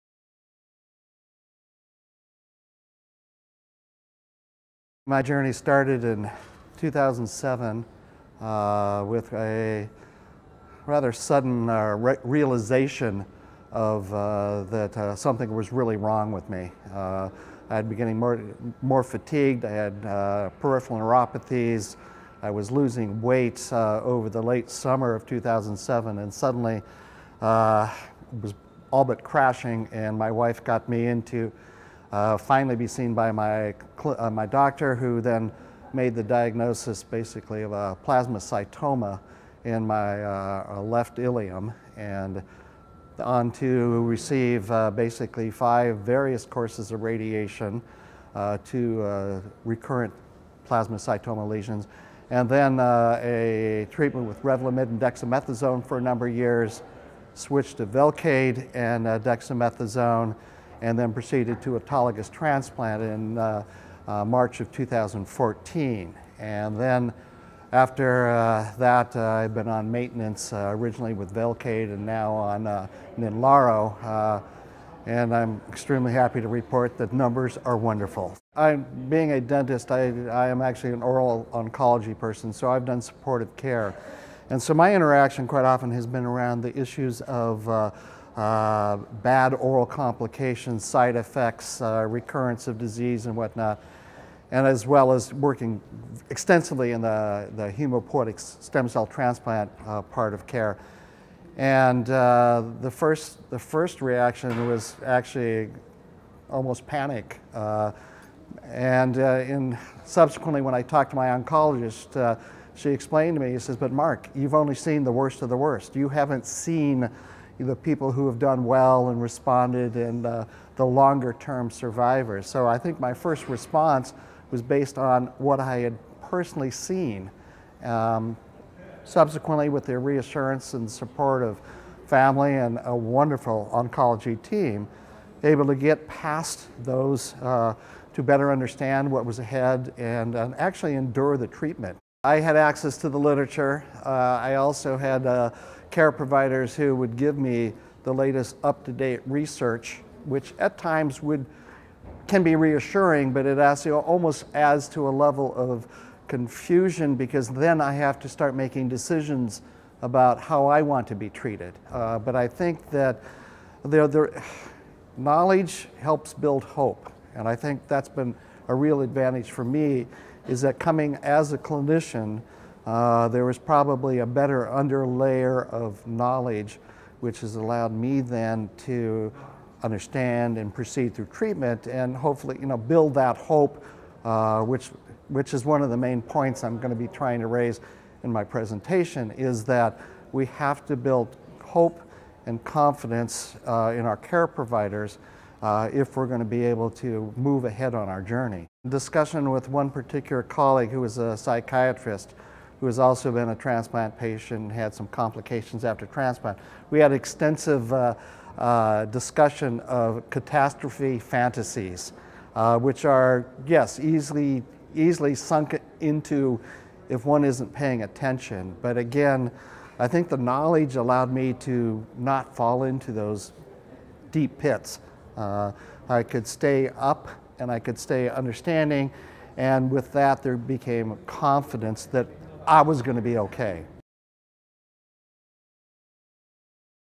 Doctor's journey through Plasmacytoma treatment
Mark Schubert, DDS, MSD of Seattle Care Alliance, Seattle, WA provides an overview of his journey through plasmacytoma treatment. With symptoms beginning in 2007, he mentions experiencing fatigue, peripheral neuropathies and weight loss, before officially being diagnosed with plasmacytoma in his left ilium. He then provides a brief overview of the various forms of treatment received: 5 courses of radiation, a treatment with lenalidomide and dexamethasone for a number of years, a switch to bortezomib and dexamethasone and then preceded to an autologous transplant in March 2014. Following that process, he was maintained with bortezomib and is now taking ixazomib. He reports feeling optimistic with the progress so far. Being a dentist, Dr. Schubert explains his experience working in supportive care, for oral oncology. He mentions knowledge surrounding the issues of bad oral complications side effects and a recurrence of disease that patients may experience. This had led to him feeling a sense of panic as a first reaction to his own diagnosis. However, he emphasizes that with the reassurance and support of family and a great oncology team, he was able to better understand and endure treatment. Having access to the literature, as well as care providers who would provide him with the latest up to date research, Dr. Schubert expresses feeling reassured, however at times a level of confusion with regards to making decisions on how he would like to be treated. Dr. Schubert concludes the interview with a takeaway message he plans to raise in his presentation being that patients have to build hope and confidence in their care providers, if they are going to be able to proceed on their treatment journey. Recorded at the Multinational Association of Supportive Care in Cancer (MASCC) and International Society of Ocular Oncology (ISOO) 2016 Annual Meeting on Supportive Care in Cancer held in Adelaide, Australia.